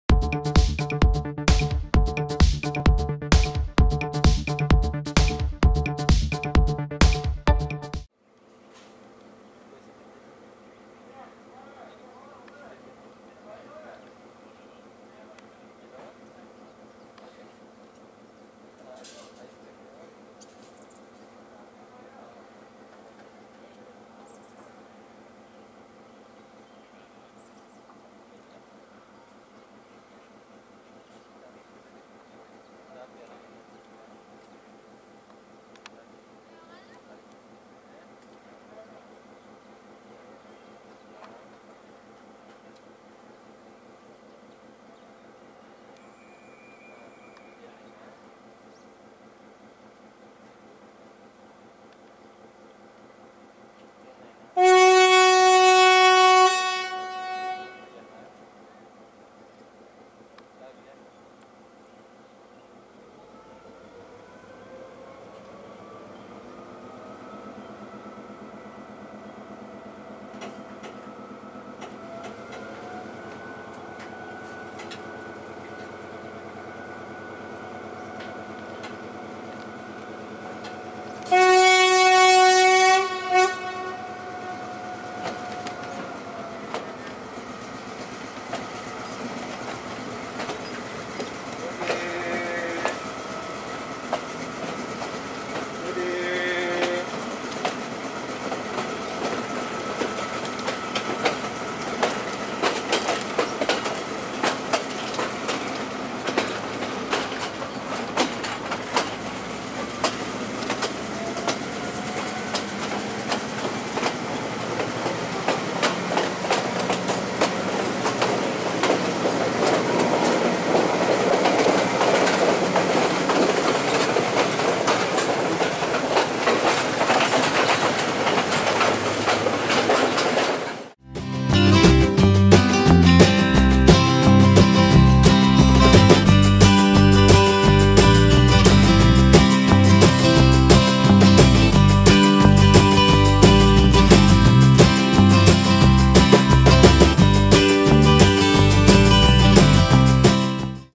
Rare Moment! Train Pulling the Trolley in Sri Lanka Railways
This is a rare moment in Sri Lanka in which the train is pulling a manual trolley by coupling it to the observation saloon.